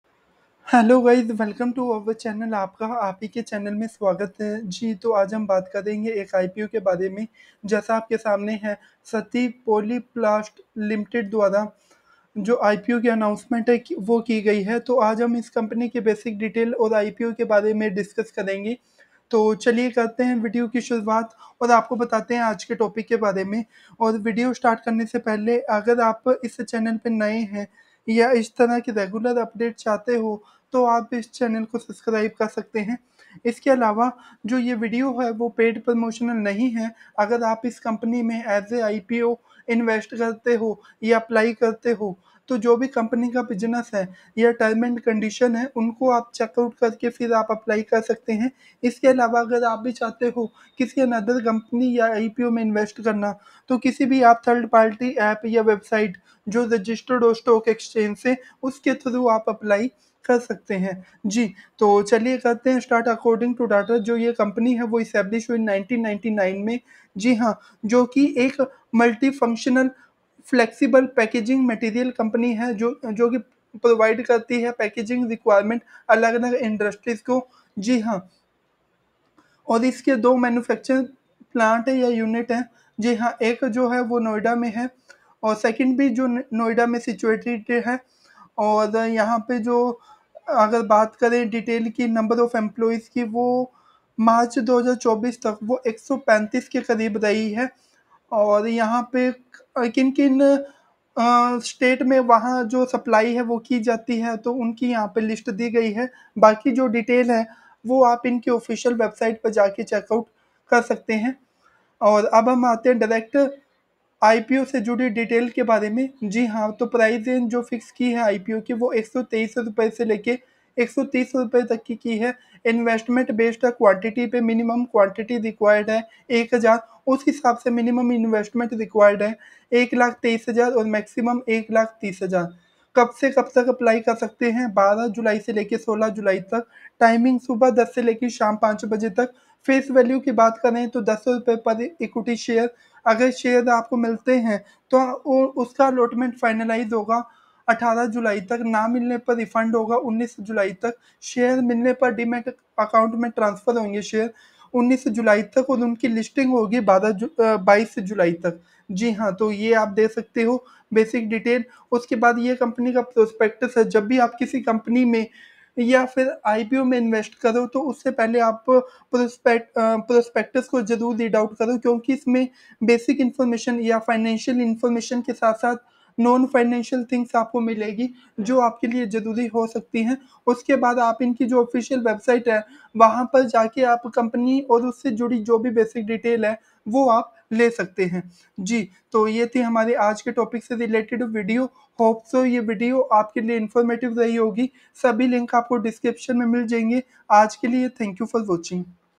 Sati Poly Plast Limited (SPPSL) SME IPO detail 2024-SearchMe (All-in-one-dunia)ipo
सती पॉली प्लास्ट लिमिटेड - एसएमई आईपीओ: जीएमपी, सदस्यता स्थिति, आवेदन तिथि, समय, निवेश और पूर्ण विवरण (Sati Poly Plast Limited - SME IPO: GMP, Subscription Status, Application Date, Timings, Investment & Full Details)